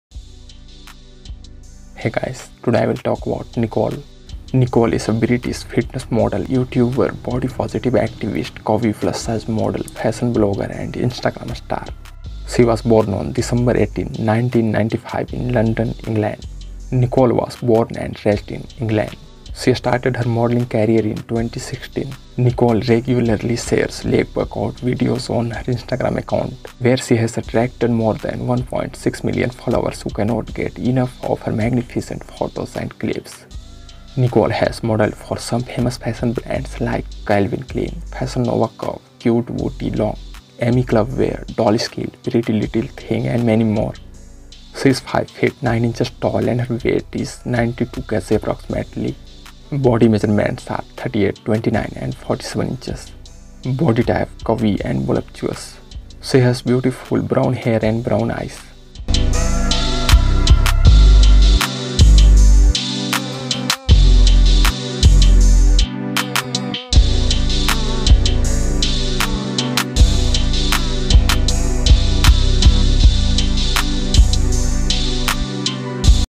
Nicole Biography | British Fitness Model, Curvy Plus Size Model Wiki
Nicole is a British Fitness Model, YouTuber, Body Positive Activist, Curvy Plus-size Model, Fashion Blogger and Instagram Star.

Sources:
Instagram and Different online resources.